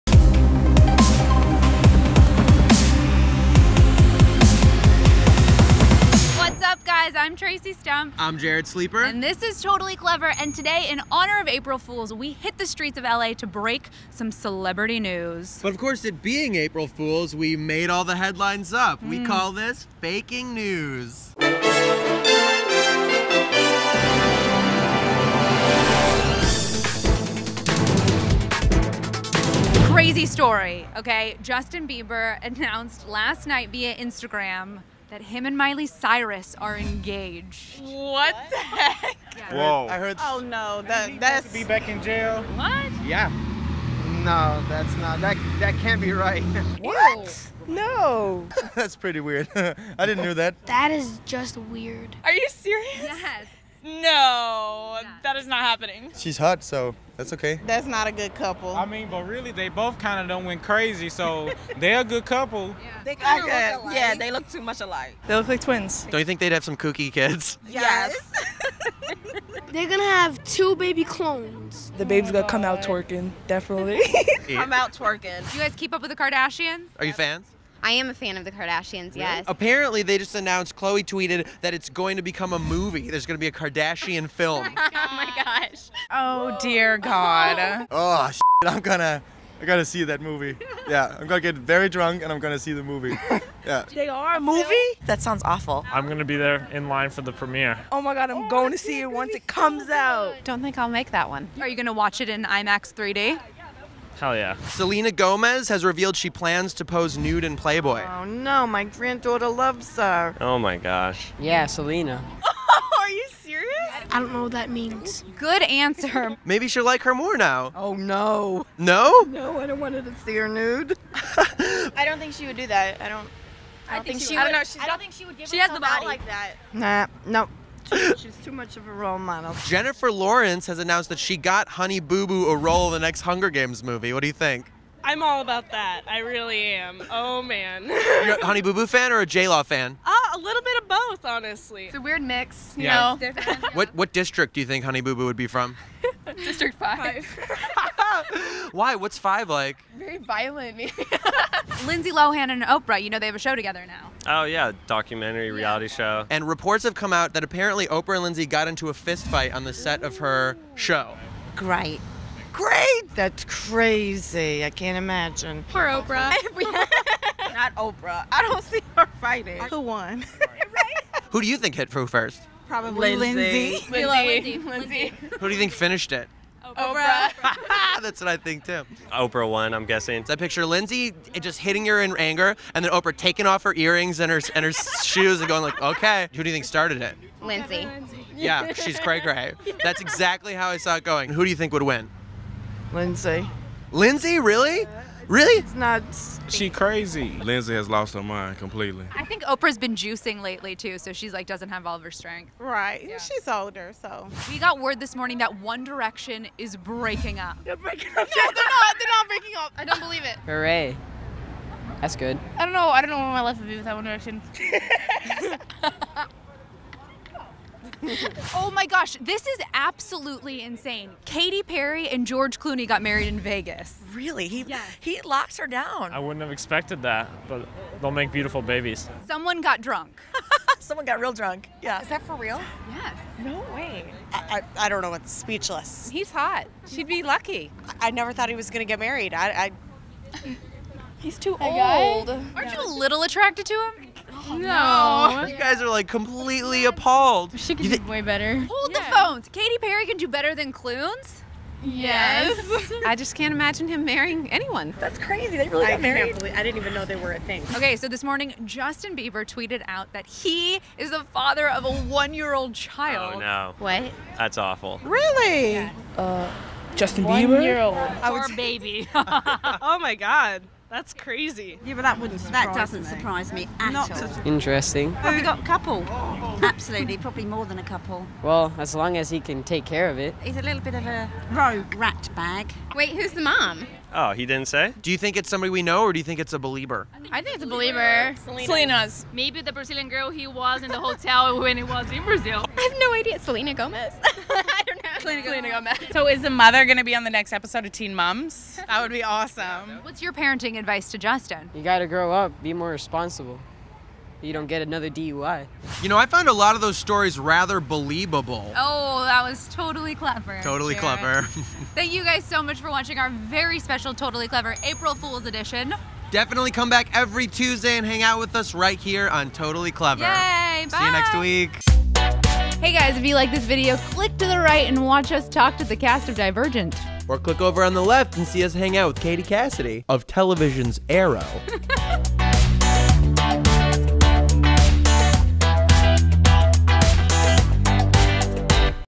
Justin Bieber Engaged and One Direction Breaking Up? - Totally Clevver
Breaking News! Justin Bieber and Miley Cyrus are engaged, Katy Perry married George Clooney in Vegas, and One Direction is breaking up! Traci and Jarrett hit the streets of LA to deliver some shocking celebrity headlines to the people -- how will they react when they hear the news?

Tweet the Hosts!
Jarrett Sleeper @jarrettsleeper
Traci Stumpf @tracistumpf